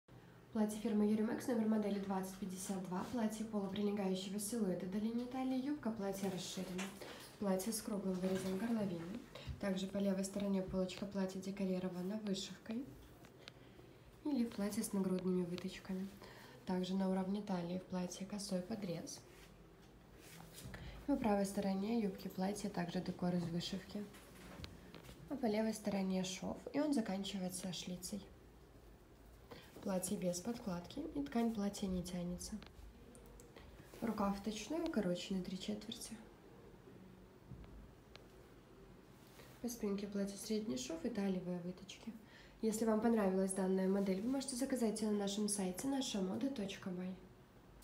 Платье : фирмы Jurimex. Номер модели: 2052
Размеры: 52, 54, 56

Цвета: Как на фото

Рост: 164 см

Состав ткани: полиэстер – 62% вискоза – 36% эластан – 2%

Длина изделия, см: 108